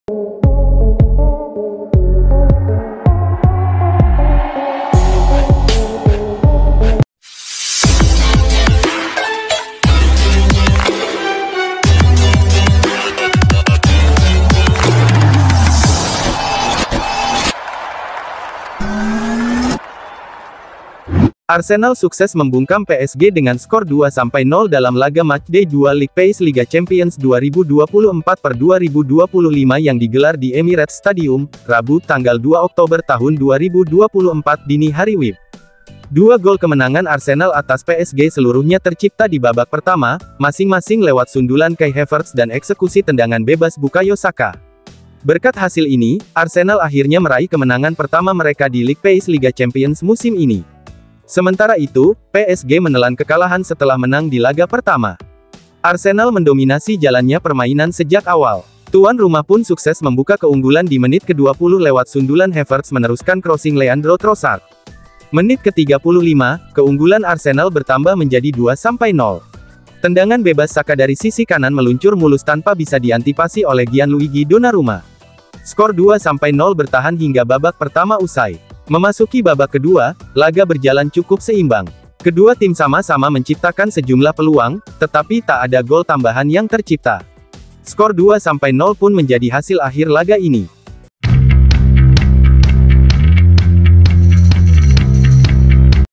HASIL ARSENAL VS PSG | ARSENAL BUNGKAM PSG 2-0
Arsenal sukses membungkam PSG dengan skor 2-0 dalam laga matchday 2 League Phase Liga Champions 2024/2025 yang digelar di Emirates Stadium, Rabu (2/10/2024) dini hari WIB.